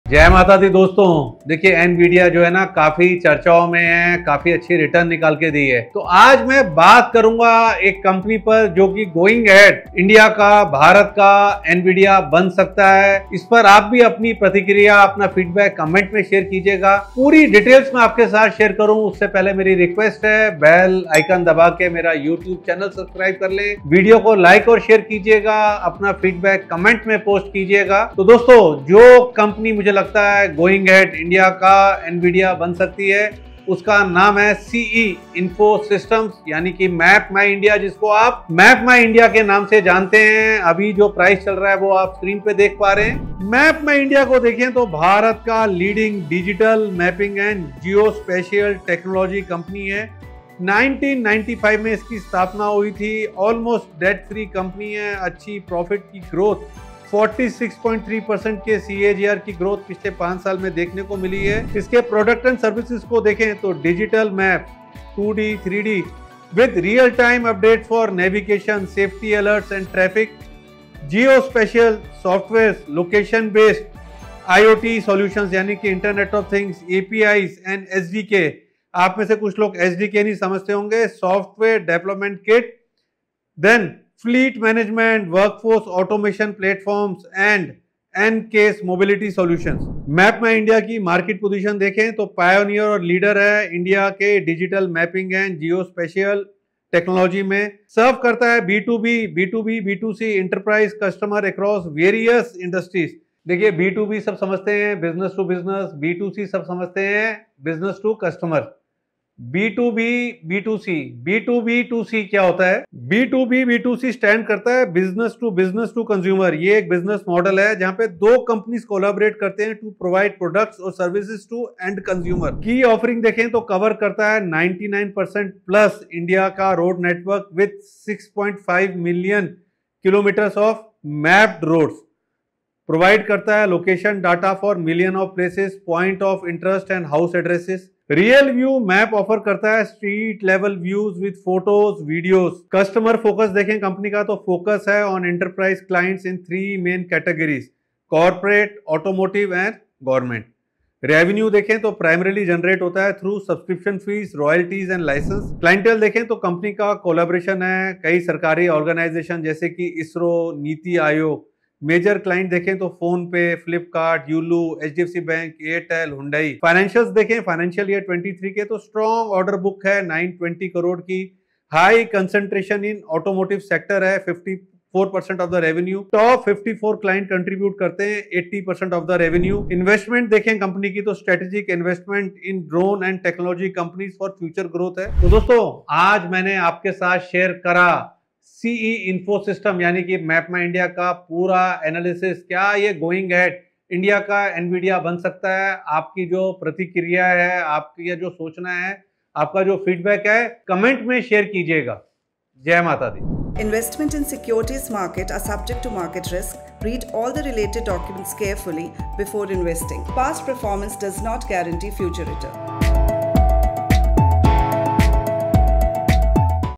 India's Nvidia ? I Rakesh Bansal
In this video, we dive deep into MapmyIndia, a company you might not have heard of but is quietly revolutionizing India's tech landscape. We'll break down their fascinating business model, showcase their innovative products, and explore the key investments driving their growth.
The NVIDIA Question: Could MapmyIndia become the "NVIDIA of India"? We'll analyze the potential parallels and discuss the factors that could propel this company to even greater heights.